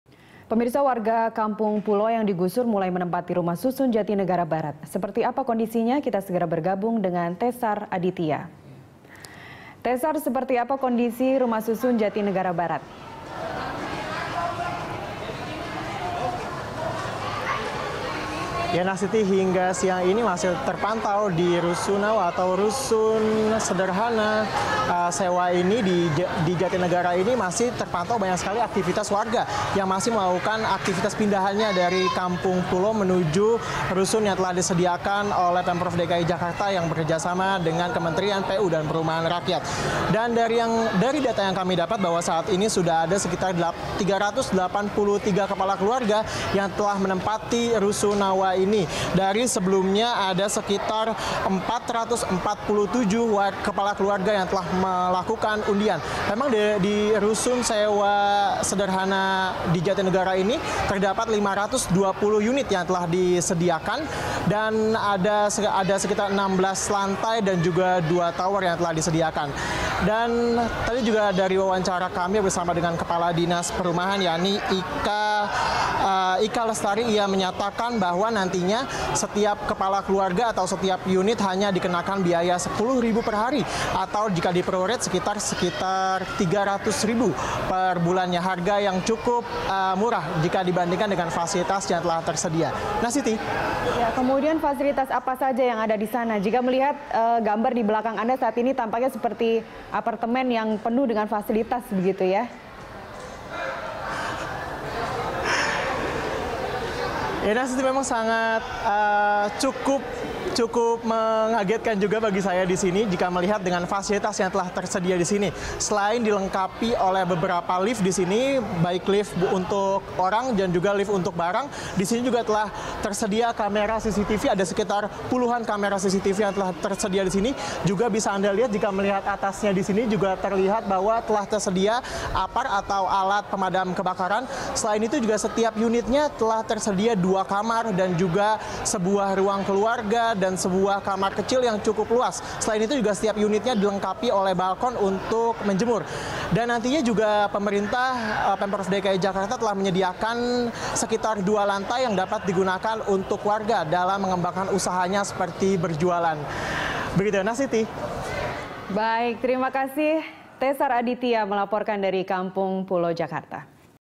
Warga Kampung Pulo Pindah ke Rusunawa
Aktivitas warga Kampung Pulo, Jakarta Timur, pindah ke Rusunawa Jatinegara Barat masih berlangsung, Sabtu (22/8/2015). Ada 383 KK menempati Rusunawa Barat.

Facebook.com/BeritaSatuTV
Youtube.com/BeritaSatu